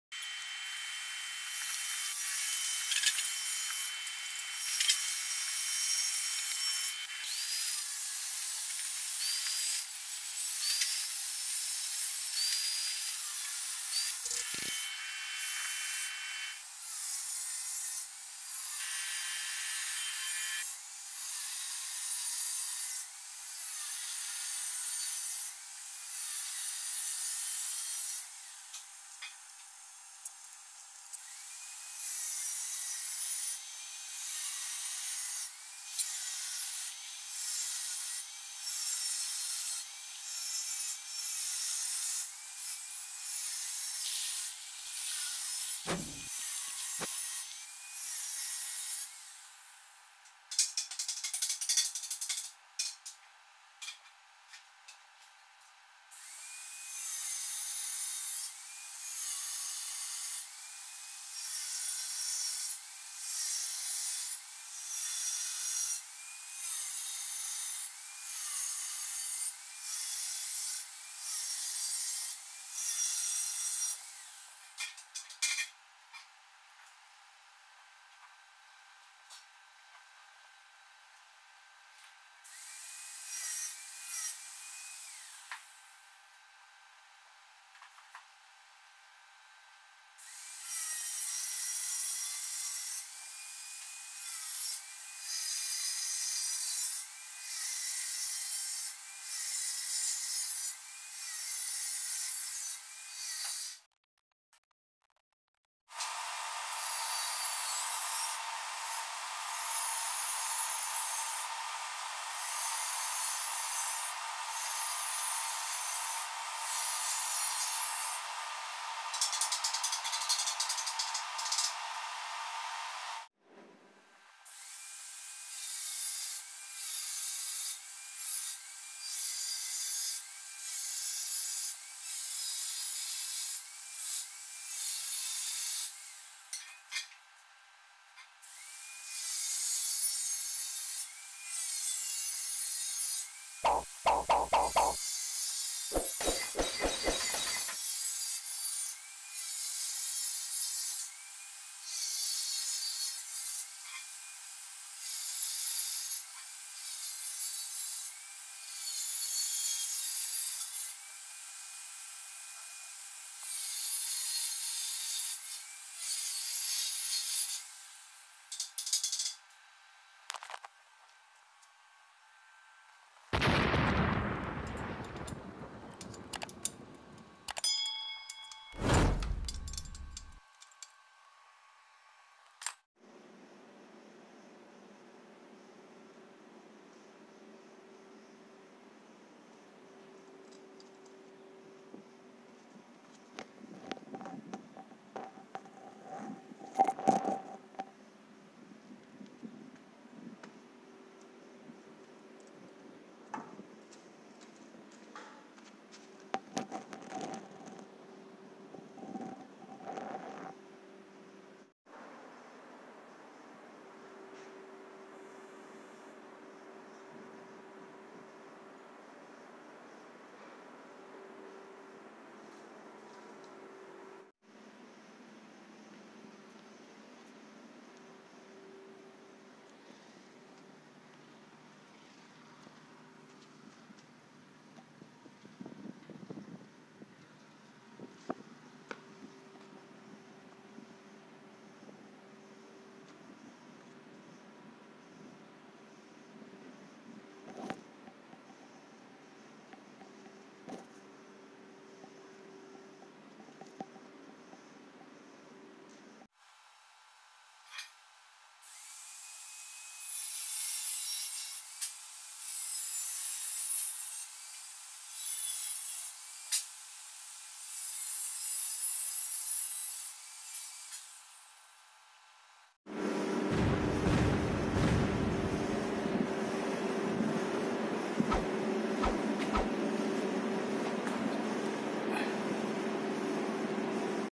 How to make Iran rack
Nadara workshop channel me phir se swagat hai,
       2"×1.1/2" square pipe me rack banayenge length 32'×4'